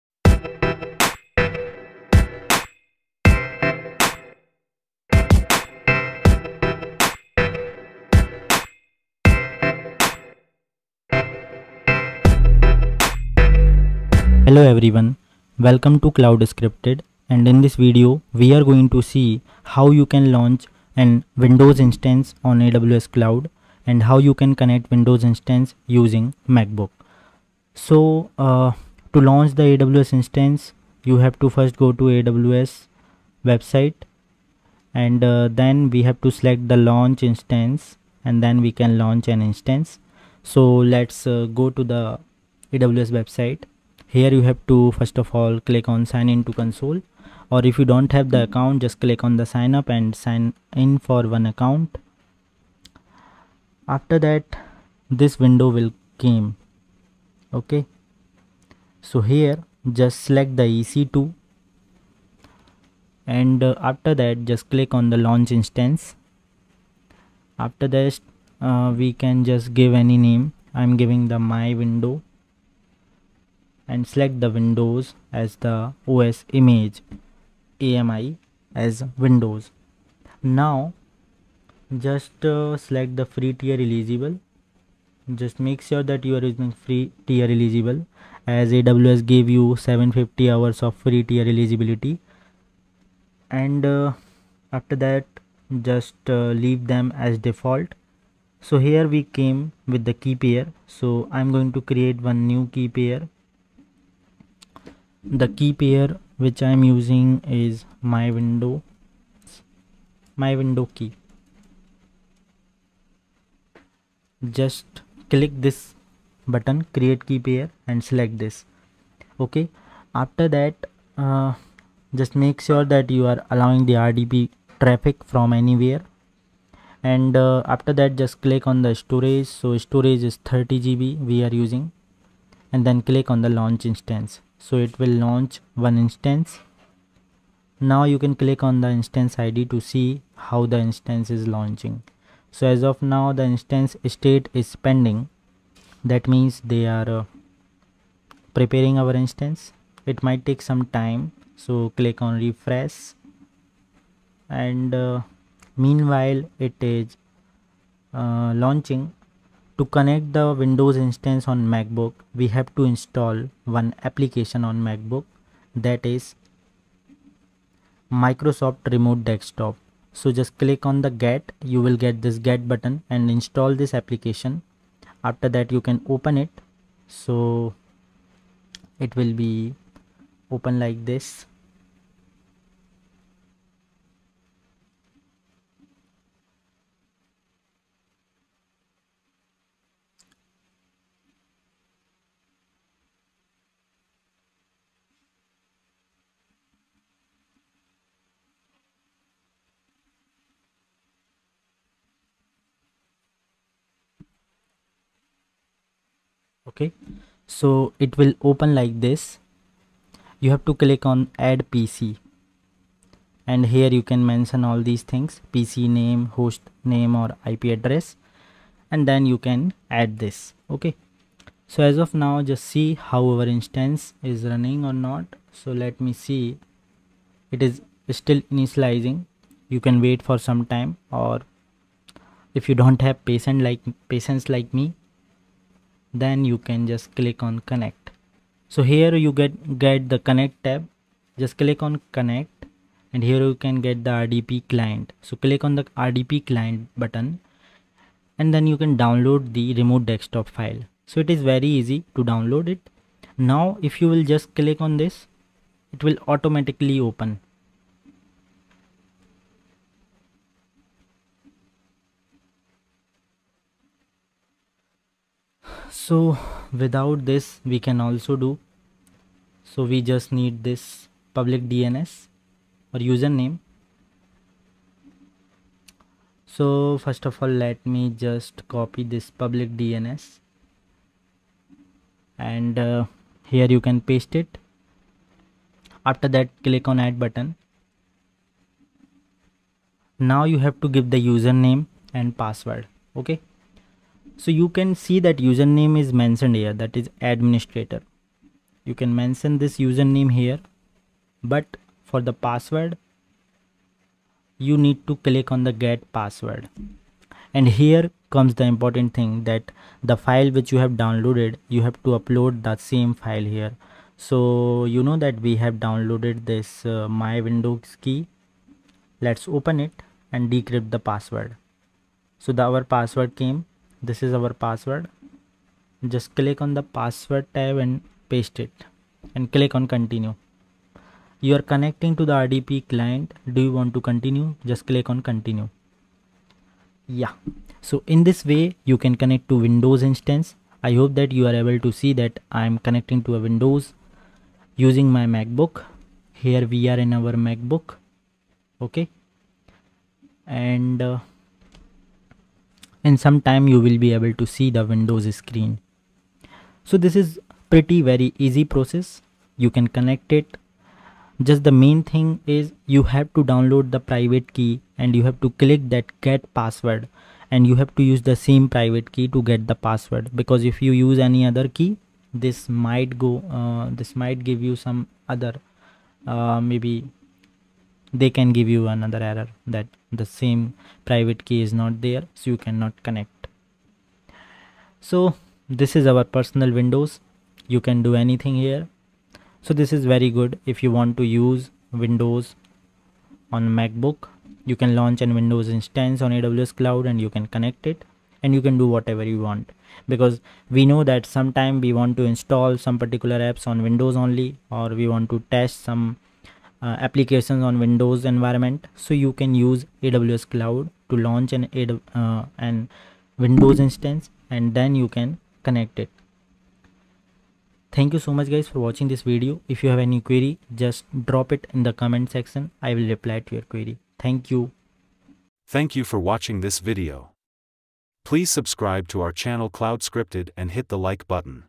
AWS EC2: Launch Windows Instance and Connect from Mac | CloudScripted Tutorial
Welcome to CloudScripted, your go-to channel for all things cloud-related! In this step-by-step tutorial, we'll guide you through the process of launching a Windows EC2 instance on AWS and seamlessly connecting to it from your Mac computer. Whether you're a cloud novice or an experienced user, this tutorial will equip you with the skills you need.

🔥 What You'll Learn:

Launching a Windows EC2 Instance on AWS
Connecting to Your EC2 Instance Using macOS
No more guessing or confusion – we've got you covered. Follow along with our expert instructions and take your cloud skills to the next level.

🌐 Dive Deeper: Full Tutorial Blog Post

Got questions or need assistance? Drop them in the comments below – we're here to help you succeed in the cloud world!

Please note: The steps and interfaces shown in this tutorial are based on the state of AWS and macOS at the time of recording. User interfaces may have changed since then.